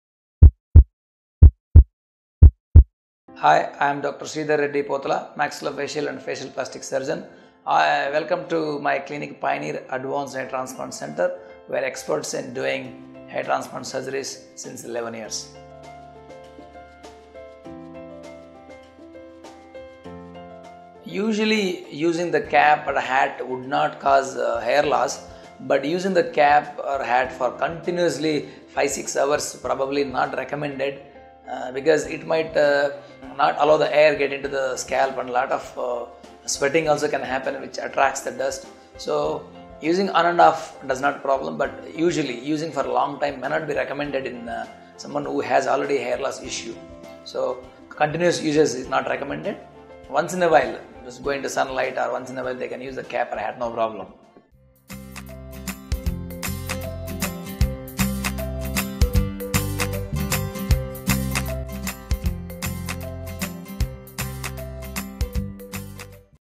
Can hats lead to hair fall after undergoing Hair Transplantation? - Dr. Sreedhar Reddy Pothula
Usually using the cap or a hat would not cause hair loss. It is not recommended using continuously for 5 to 6 hours because it might not allow the air get into the scalp and a lot of sweating also happens which attracts the dust. So using on and off may not be a problem. But using for long time may not be recommended in someone who has already hair loss issue. So continuous use is not recommended. Once in a while, just going into sunlight or what they can use the cap.